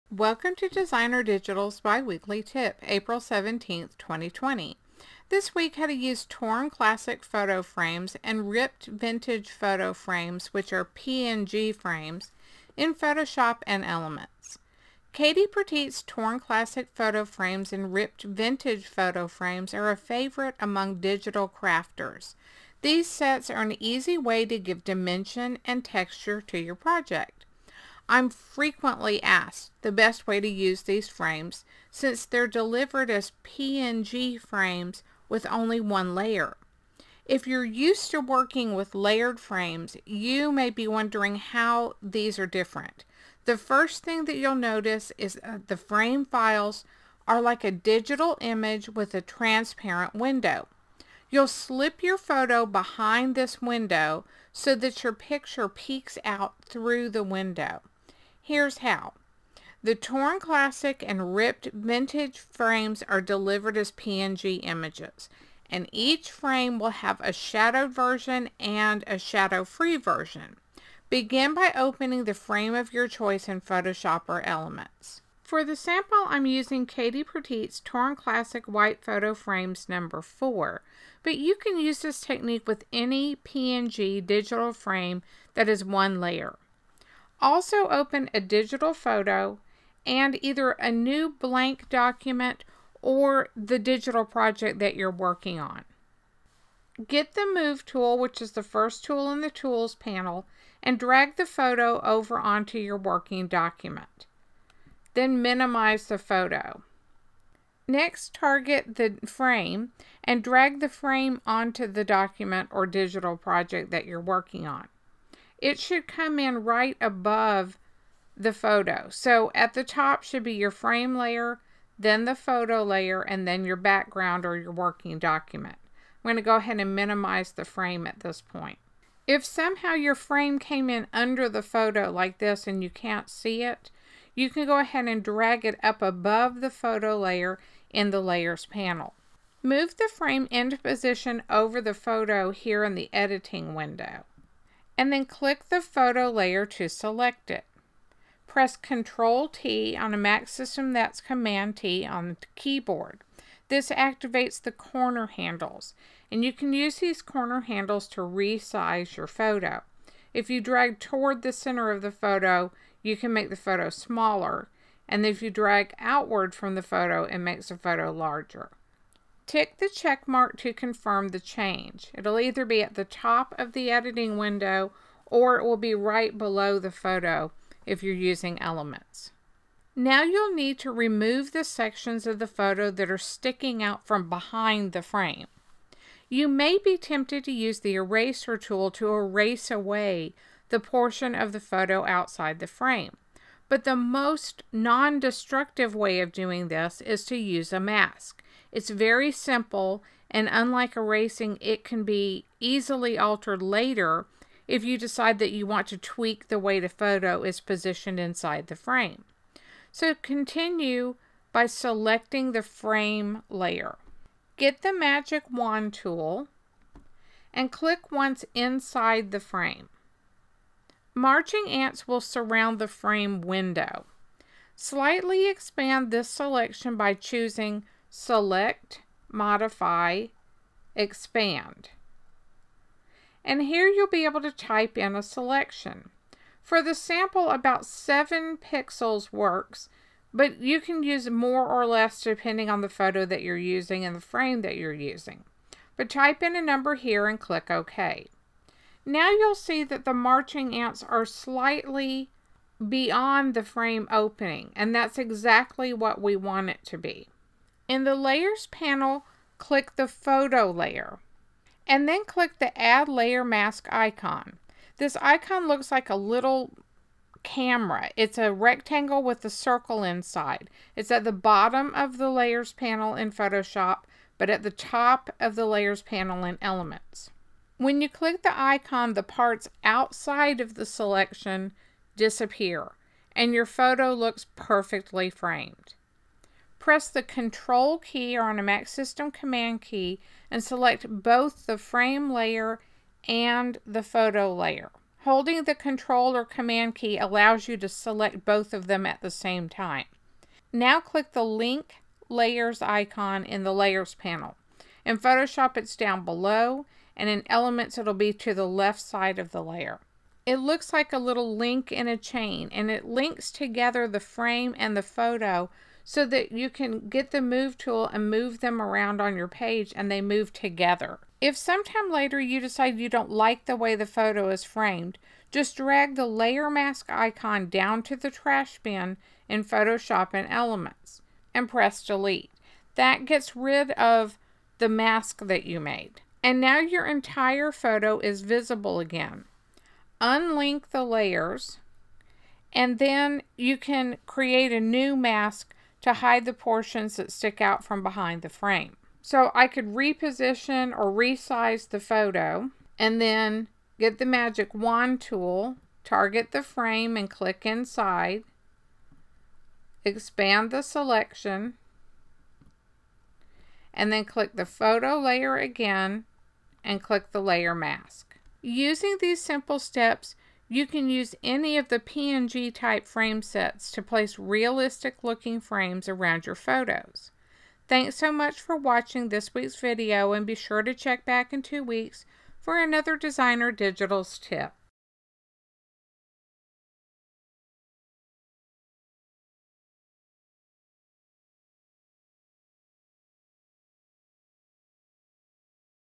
How to Use Torn and Ripped Photo Frames (PNG Frames) in Photoshop and PSE
Digital Scrapbooking tip from KatiePertietDesigns using Adobe Photoshop or Adobe Photoshop Elements. This week: How to Use Torn and Ripped Photo Frames (PNG Frames) in Photoshop and PSE.